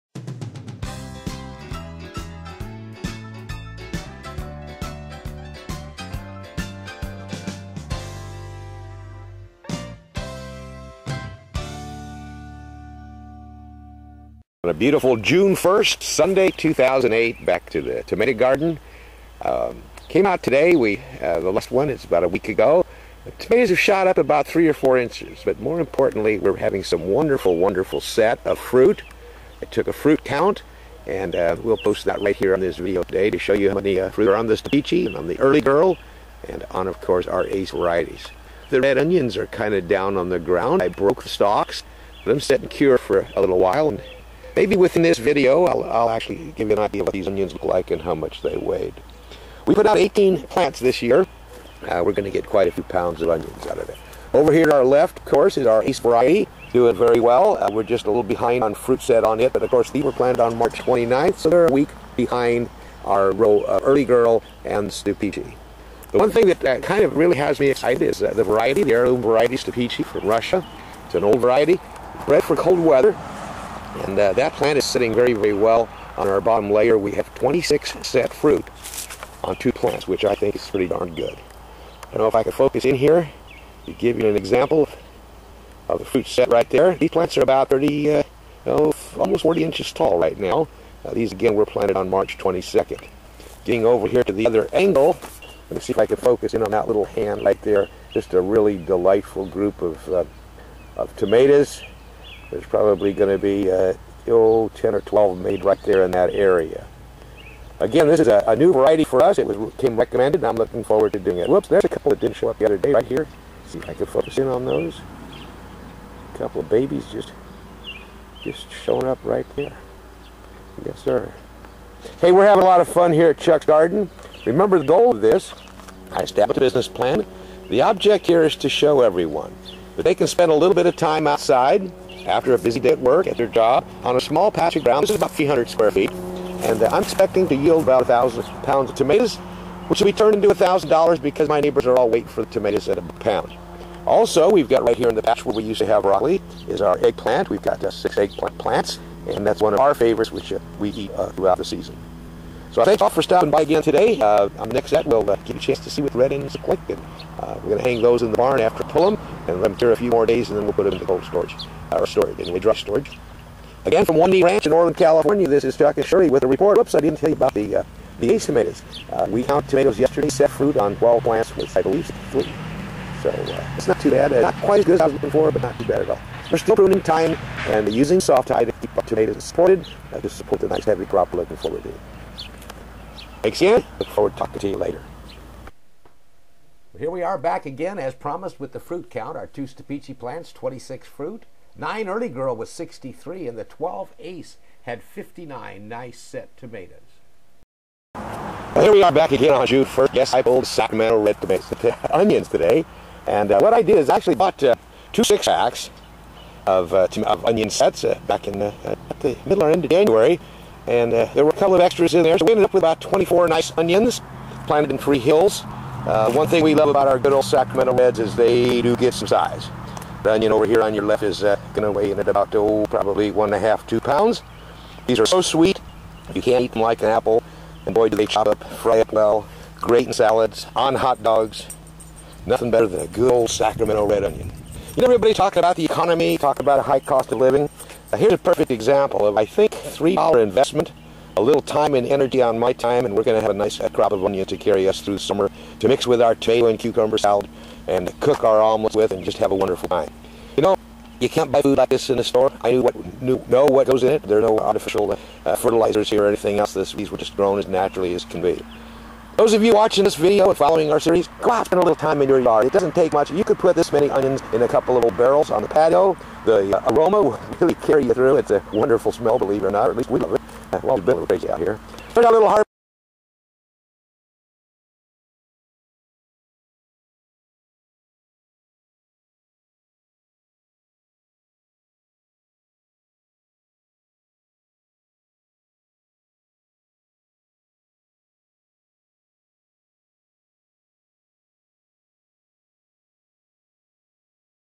Part 9 , Week 9 Growing tomatoes for Health and Wealth
A visit back to the 24 Tomato plants planted in March. Good fruit set and more coming. Also the results of the Red Onion Project